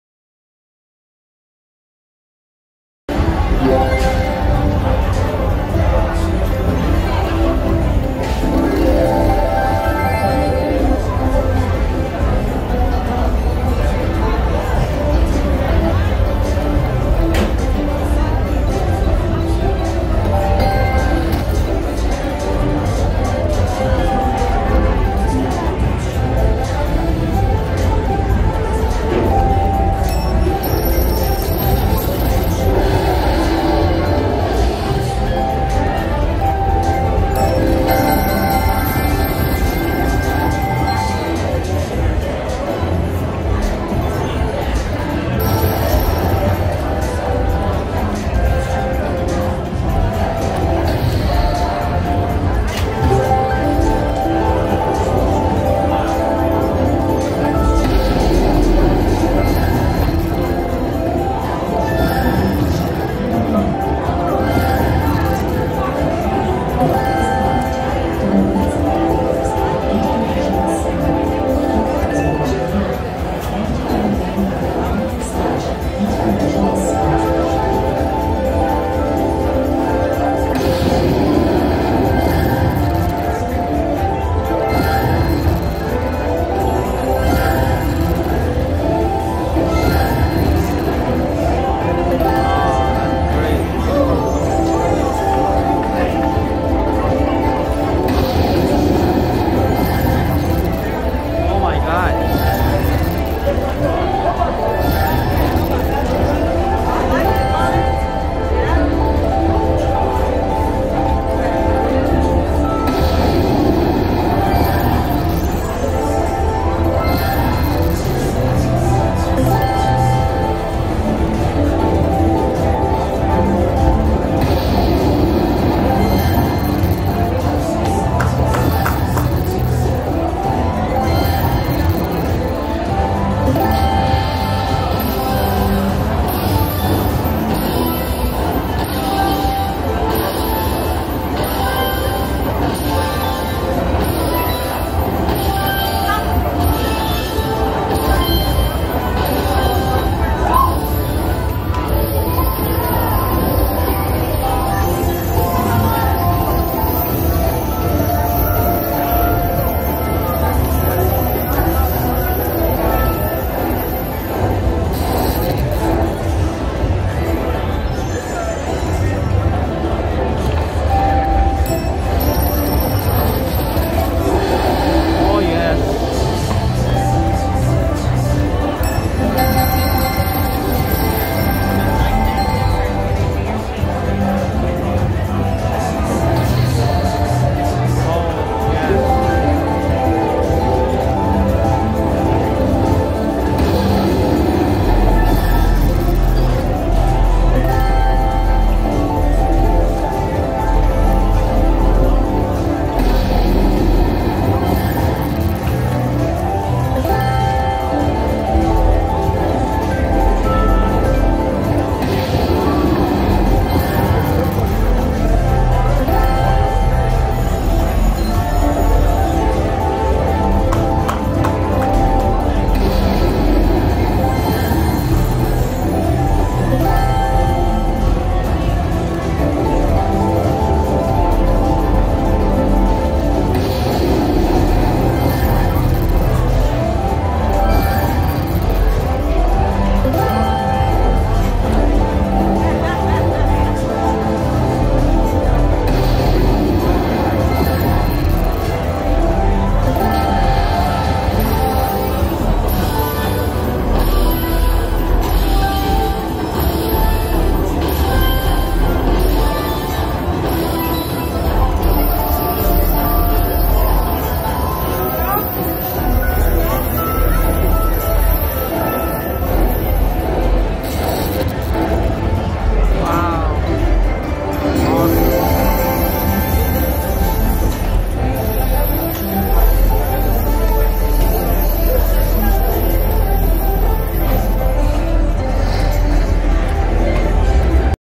Big win on Mighty Cash Ultra. 5c Denom. Free spin and bonus features! 슬랏
Playing Mighty Cash Ultra at MGM resort & Casino. I guess this is a newer version of Mighty Cash. Super Lucky to win a free game. A golden dragon pops out of nowhere and add more credits during bonus. So excited when I saw them running around. Hope you guys be lucky and enjoy the game.